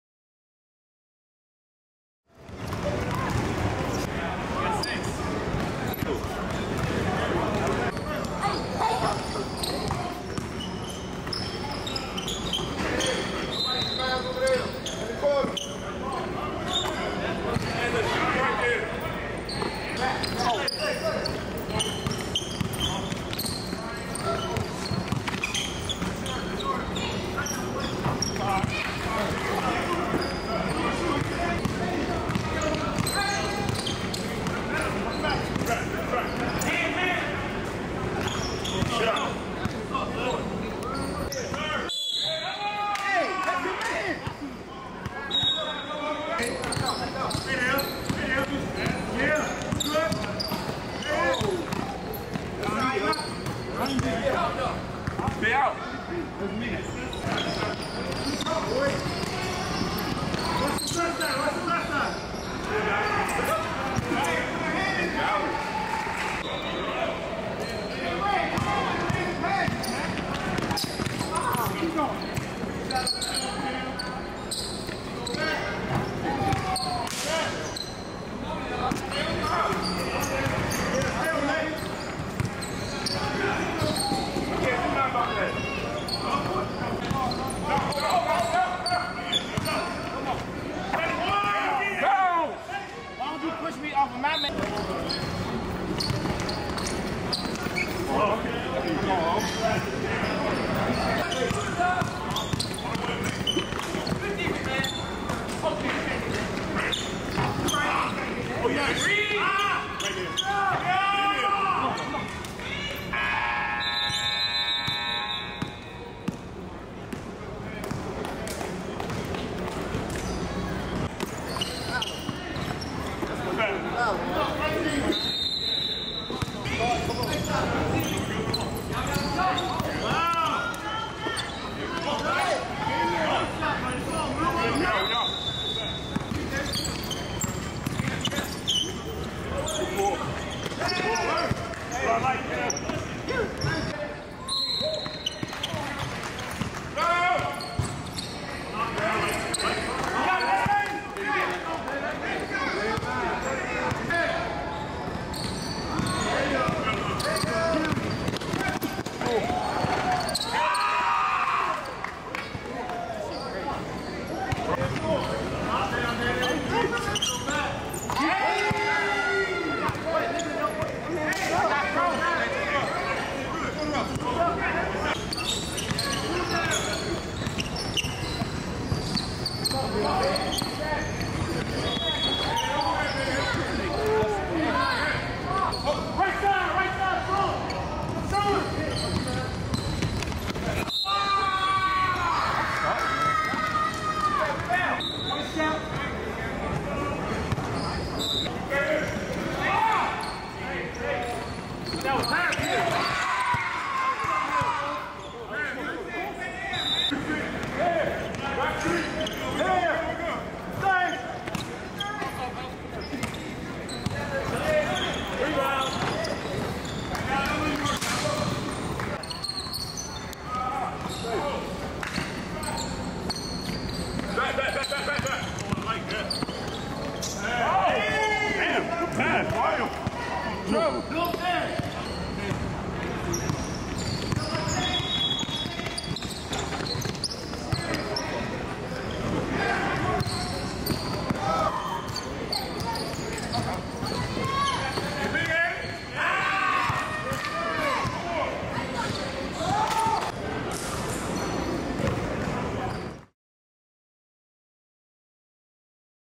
BOUNCE HOUSE vs WOLFPAC | VBFH Adult League
VB FIELD HOUSE ADULT LEAGUE
Bounce House H/L vs WolfPac

Bounce House wins 62-54

Filmed by: Matt O'mara
Edited by: @daseanwebster